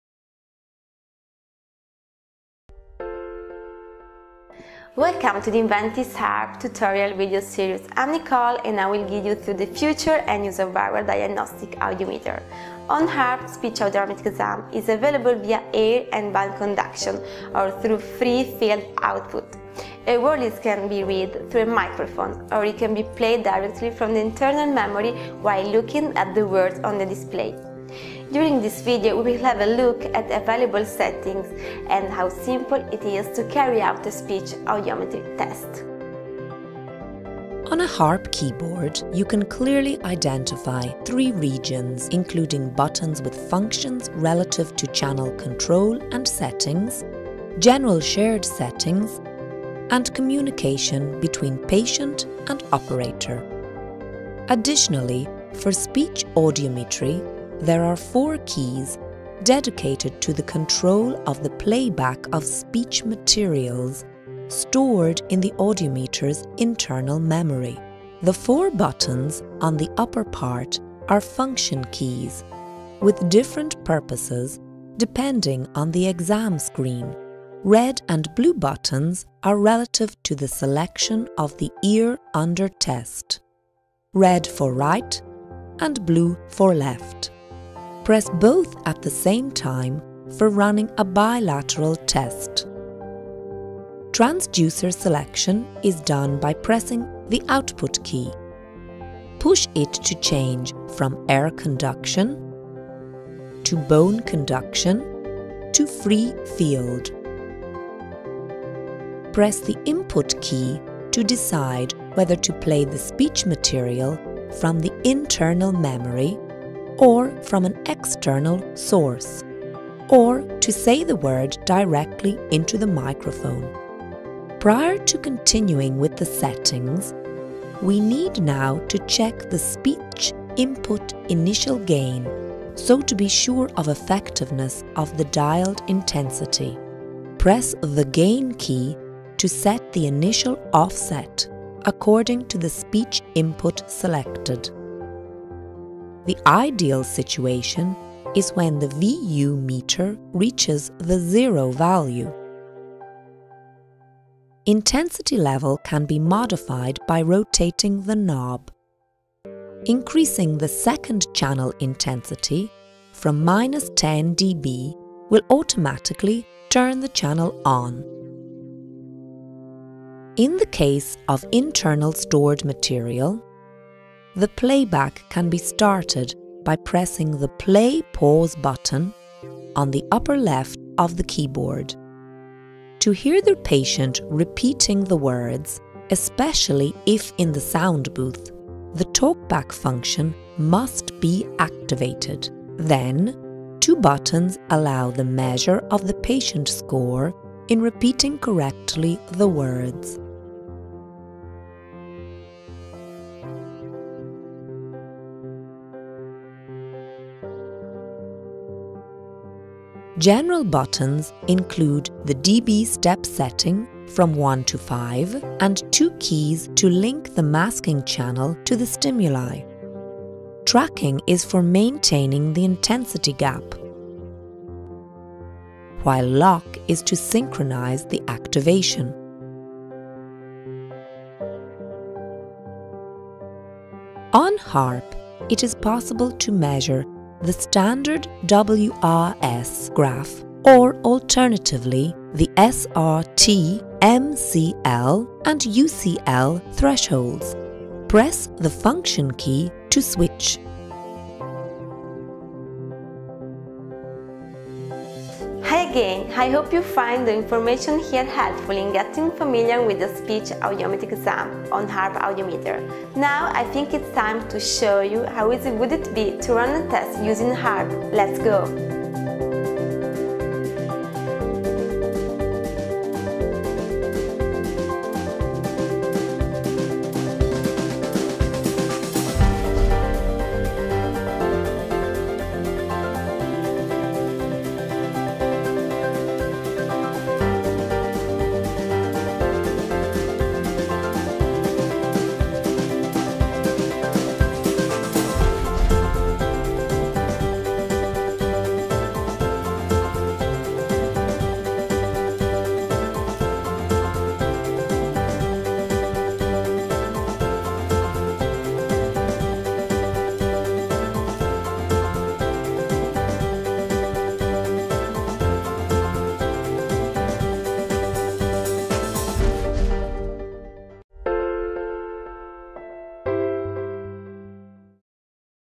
Audiómetro HARP de Inventis. Logoaudiometria
Demostración de una prueba de logoaudiometría con un audiómetro HARP de Inventis.
El uso de una memoria USB con la lista de palabras facilita enormemente la ejecución de la prueba. Adiós a los reproductores de CDs o mp3 externos!